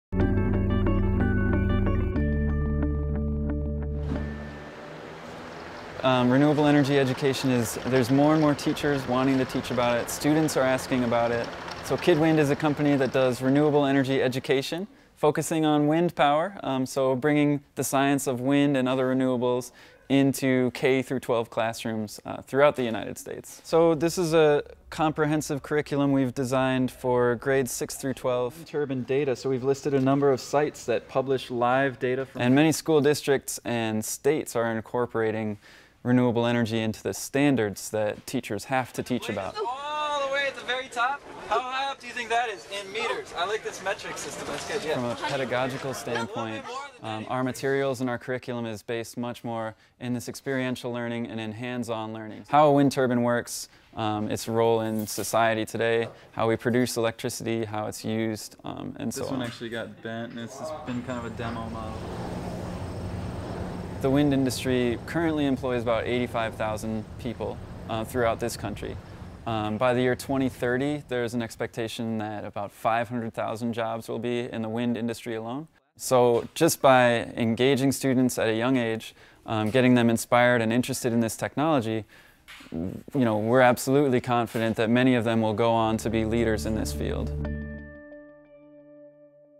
MN2020: KidWind
Educators strive to bring experiencial learning for future life skills into the classroom.  Less time sitting at a desk and more time with stimulating hands-on experimentation.  New curricula and learning standards are incorporation new disciplines in math, physics, and social sciences of wind power.
Kid Wind, a St. Paul based education company, manufactures wind turbine models for classroom learning and teacher lesson plans for in depth study.The University of Minnesota is launching a new curriculum of study focusing on wind turbine implementation and installation for future sites across the nation.